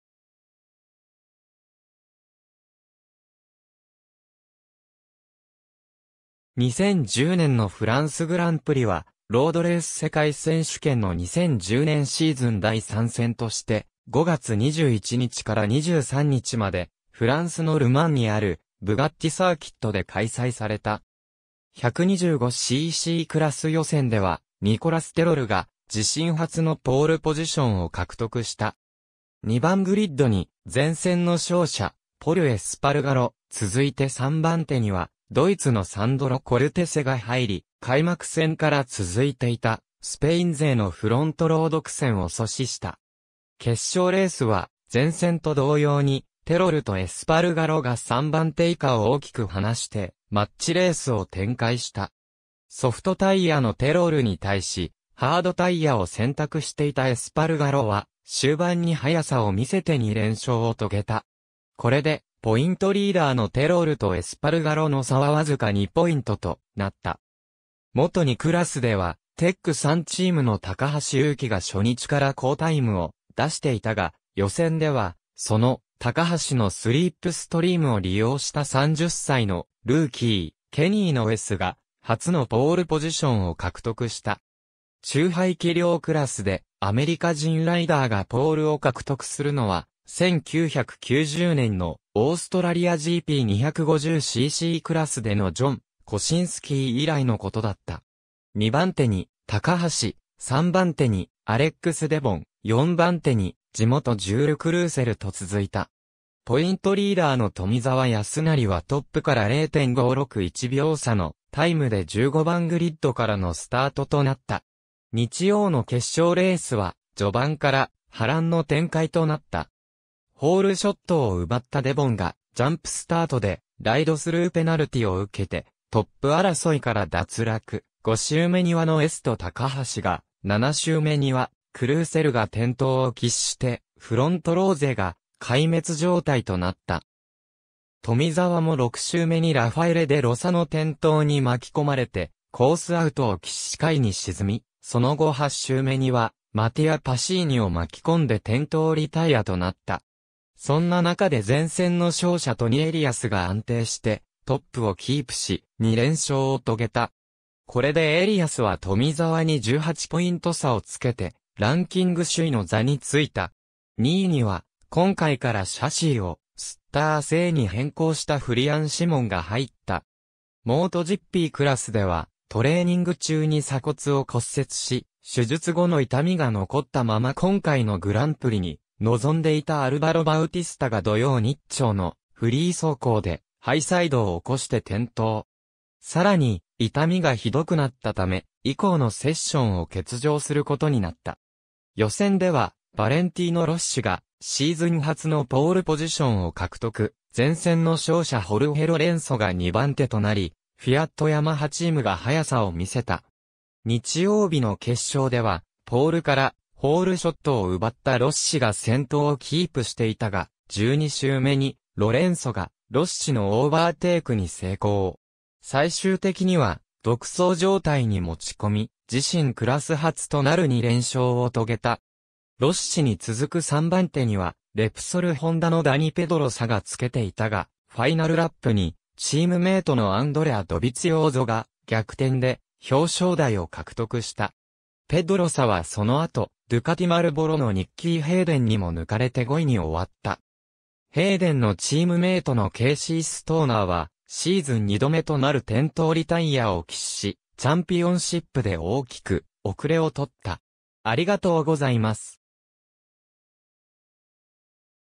2010年のフランスグランプリ (ロードレース)
フランスグランプリ_(ロードレース)
2010年のフランスグランプリ は、ロードレース世界選手権の2010年シーズン第3戦として、5月21日から23日までフランスのル・マンにあるブガッティ・サーキットで開催された。
125ccクラス予選では、ニコラス・テロルが自身初のポールポジションを獲得した。
2番グリッドに前戦の勝者ポル・エスパルガロ、続いて3番手にはドイツのサンドロ・コルテセが入り、開幕戦から続いていたスペイン勢のフロントロウ独占を阻止した。
決勝レースは前戦と同様に、テロルとエスパルガロが3番手以下を大きく離してマッチレースを展開した。
ソフトタイヤのテロルに対し、ハードタイヤを選択していたエスパルガロは終盤に速さを見せて2連勝を遂げた。
これでポイントリーダーのテロルとエスパルガロの差は僅か2ポイントとなった。
Moto2クラスでは、テック3チームの高橋裕紀が初日から好タイムを出していたが、予選ではその高橋のスリップストリームを利用した30歳のルーキー、ケニー・ノエスが初のポールポジションを獲得した。
中排気量クラスでアメリカ人ライダーがポールを獲得するのは、1990年のオーストラリアGP250ccクラスでのジョン・コシンスキー以来のことだった。
2番手に高橋、3番手にアレックス・デボン、4番手に地元ジュール・クルーセルと続いた。
ポイントリーダーの富沢祥也はトップから0.561秒差のタイムで15番グリッドからのスタートとなった。
日曜の決勝レースは序盤から波乱の展開となった。
ホールショットを奪ったデボンがジャンプスタートでライドスルーペナルティを受けてトップ争いから脱落、5周目にはノエスと高橋が、7周目にはクルーセルが転倒を喫して、フロントロウ勢が壊滅状態となった。
富沢も6周目にラファエレ・デ・ロサの転倒に巻き込まれてコースアウトを喫し下位に沈み、その後8周目にはマティア・パシーニを巻き込んで転倒リタイヤとなった。
そんな中で前戦の勝者トニ・エリアスが安定してトップをキープし、2連勝を遂げた。
これでエリアスは富沢に18ポイント差をつけて、ランキング首位の座についた。
2位には今回からシャシーをスッター製に変更したフリアン・シモンが入った。
MotoGPクラスでは、トレーニング中に鎖骨を骨折し、手術後の痛みが残ったまま今回のグランプリに臨んでいたアルバロ・バウティスタが土曜日朝のフリー走行でハイサイドを起こして転倒。
さらに痛みがひどくなったため、以降のセッションを欠場することになった。
予選ではバレンティーノ・ロッシがシーズン初のポールポジションを獲得、前戦の勝者ホルヘ・ロレンソが2番手となり、フィアット・ヤマハチームが速さを見せた。
日曜日の決勝では、ポールからホールショットを奪ったロッシが先頭をキープしていたが、12周目にロレンソがロッシのオーバーテイクに成功。
最終的には独走状態に持ち込み、自身クラス初となる2連勝を遂げた。
ロッシに続く3番手にはレプソル・ホンダのダニ・ペドロサが付けていたが、ファイナルラップにチームメイトのアンドレア・ドヴィツィオーゾが逆転で表彰台を獲得した。
ペドロサはその後ドゥカティ・マルボロのニッキー・ヘイデンにも抜かれて5位に終わった。
ヘイデンのチームメイトのケーシー・ストーナーはシーズン2度目となる転倒リタイヤを喫し、チャンピオンシップで大きく後れを取った。